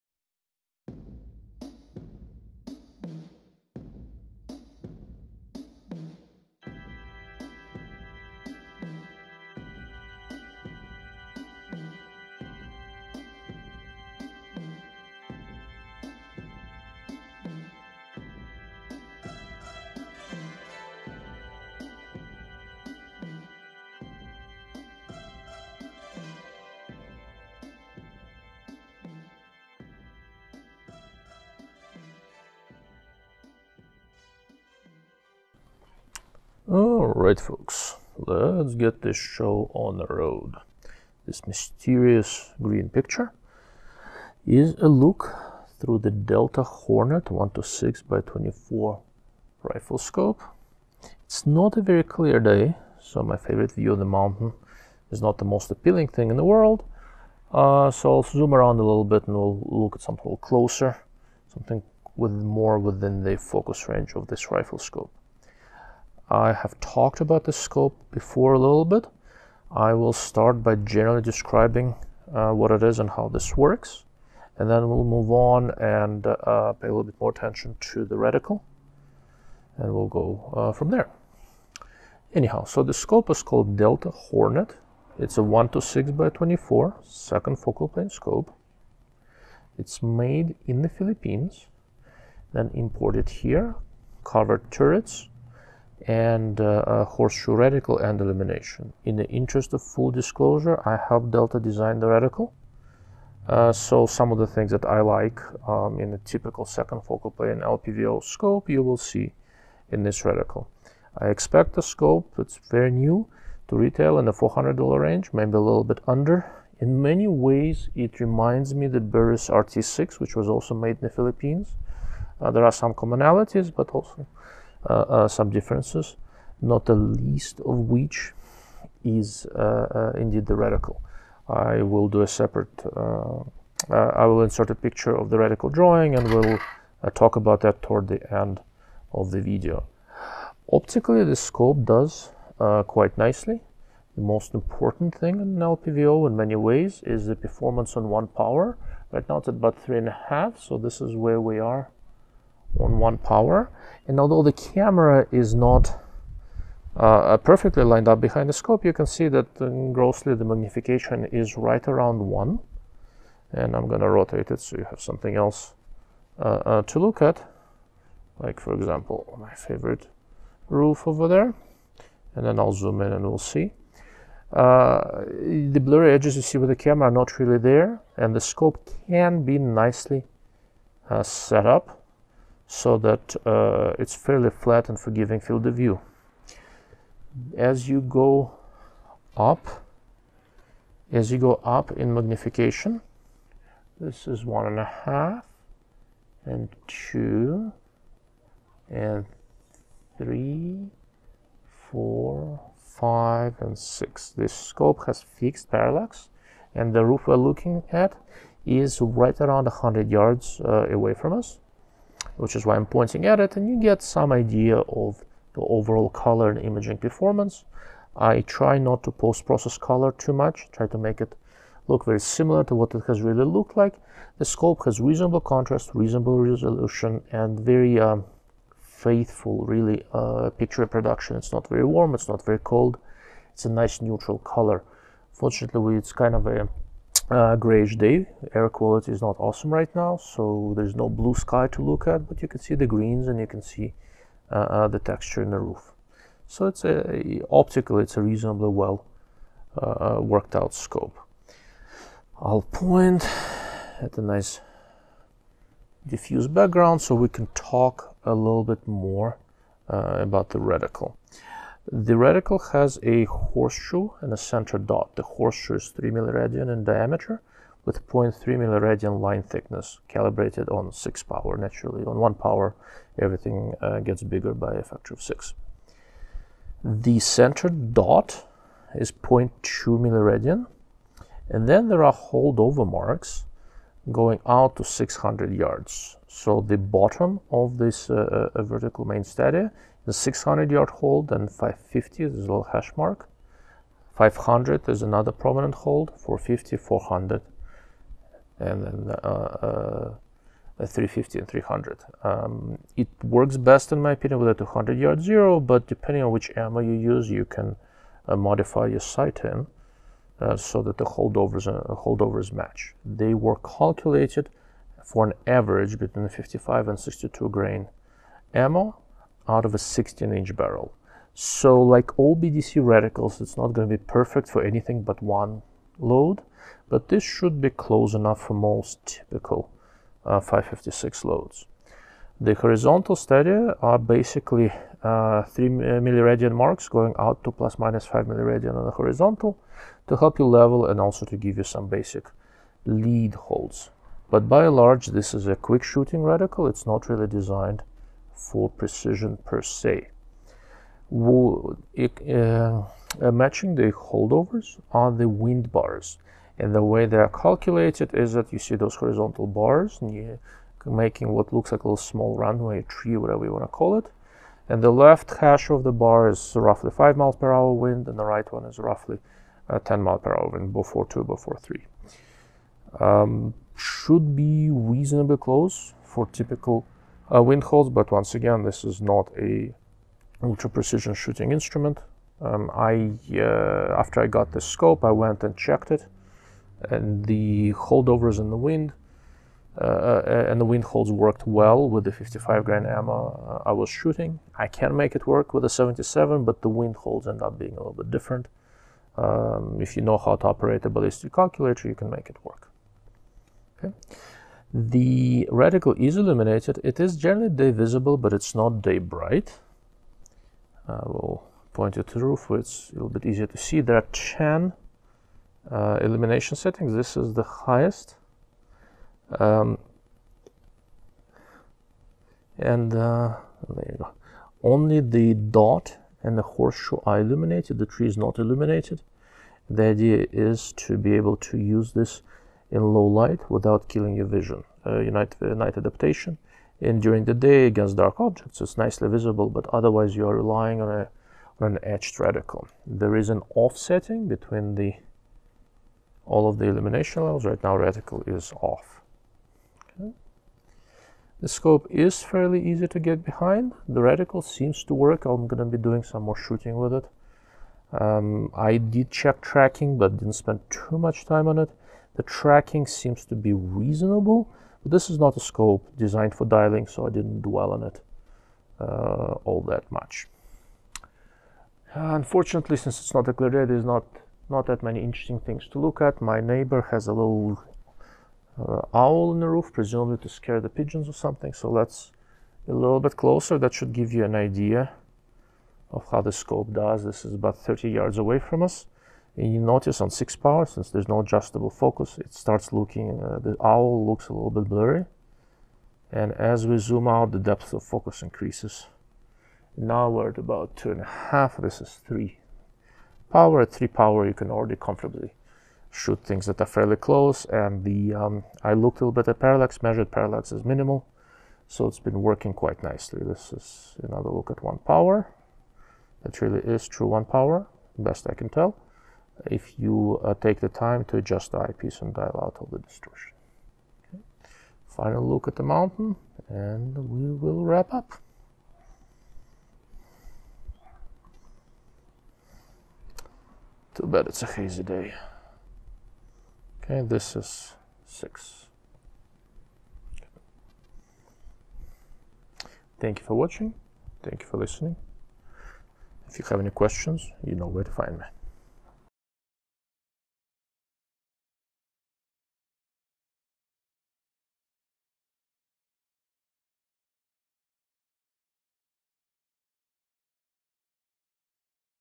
DLO Reviews: Delta Hornet 1-6x24 with D2BR Reticle
In this video we take a look through the new Delta Hornet 1-6x24 LPVO with D2BR reticle.
I start out by discussing the overall features and performance of the scope, with the reticle discussion starting at around 4:30 mark.